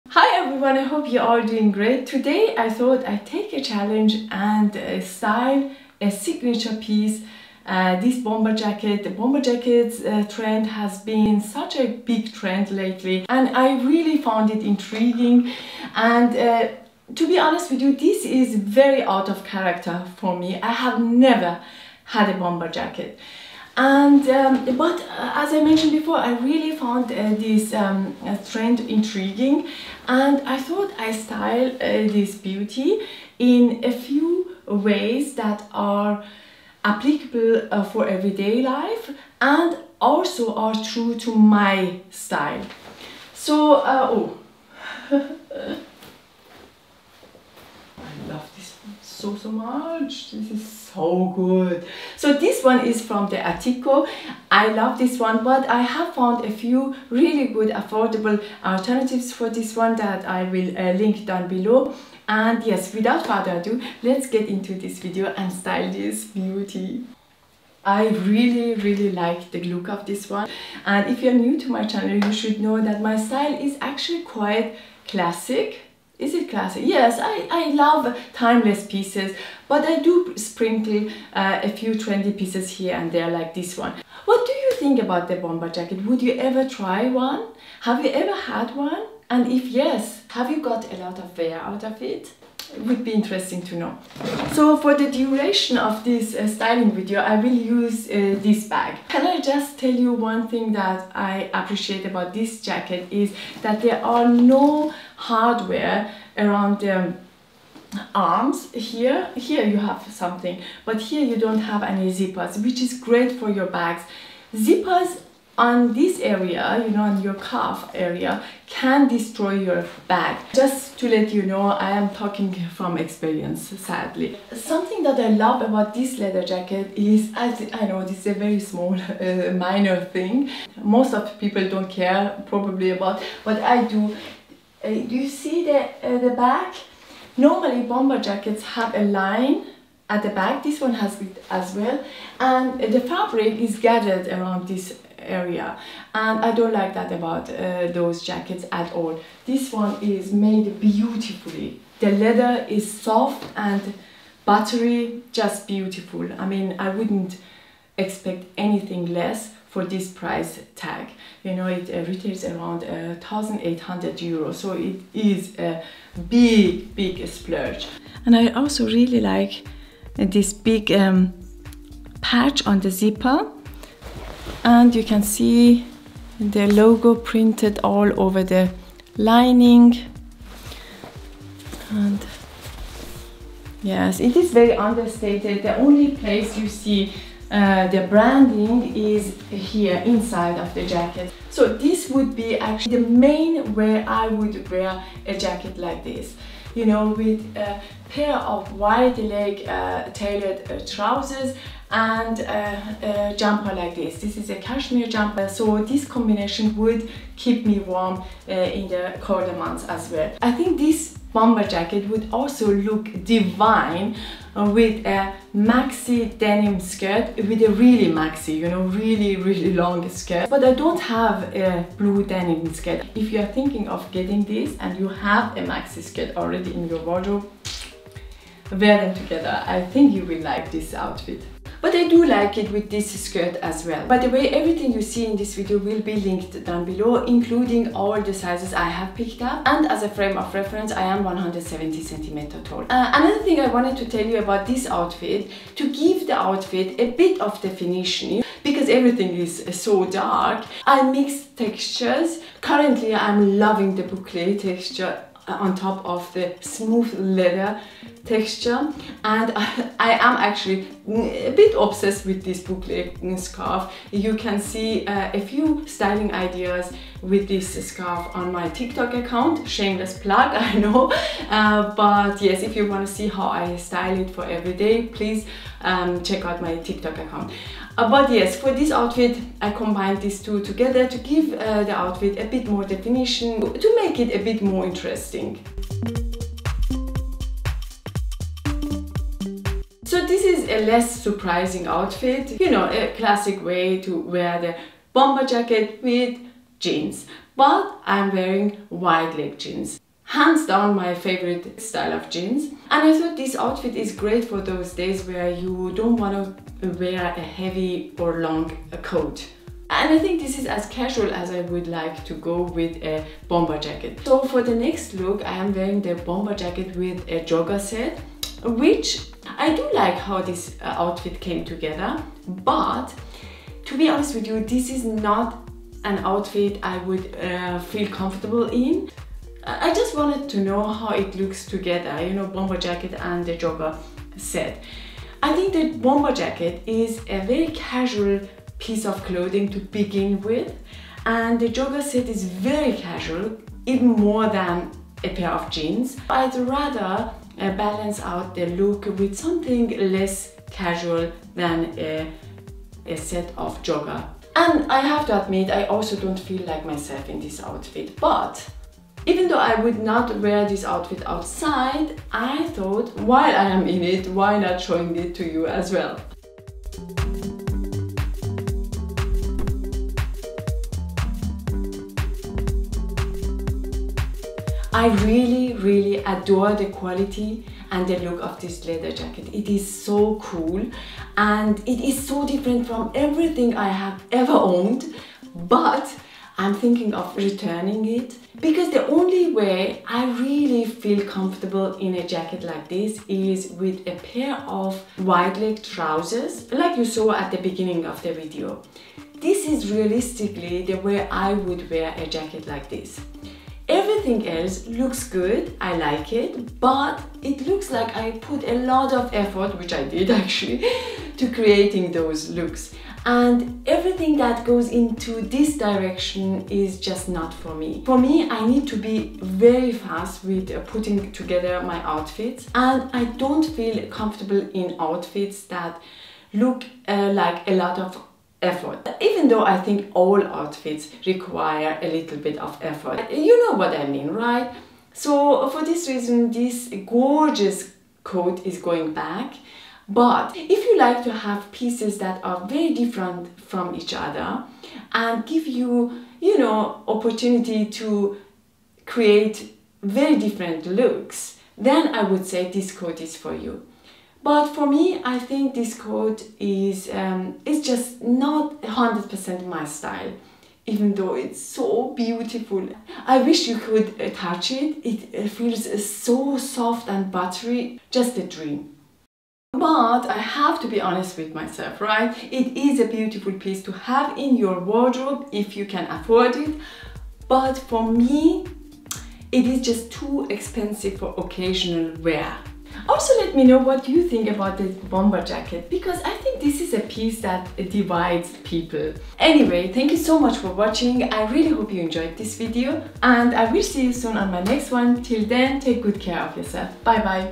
The 2023 Leather Trend | Styling The Attico Bomber Jacket With Items From Raey, Toteme, Arket, COS
The leather trend is in general going to be huge in 2023 and bomber jackets are making a big comeback. In this video I am styling the trendy leather bomber jacket by the Attico for casual but also smart occasions.

Cap (mine is by NY but I have this one on my wish list)

Mahshid